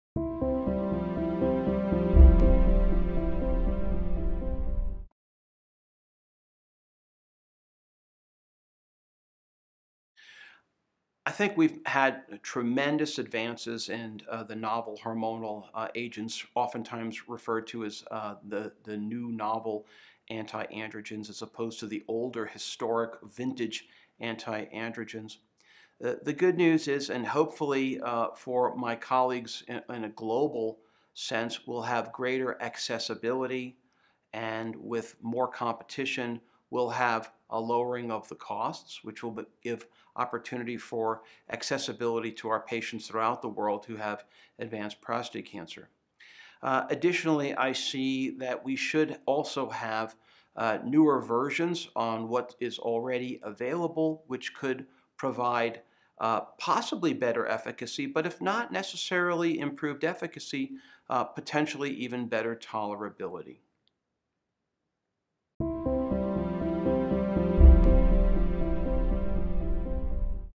Ask the Expert: What Changes Are Happening in Anti Androgen Therapy for Prostate Cancer?
Neal D. Shore, MD, FACS, Carolina Urologic Research Center, Myrtle Beach, South Carolina, discusses the changes happening in anti-androgen therapy for prostate cancer.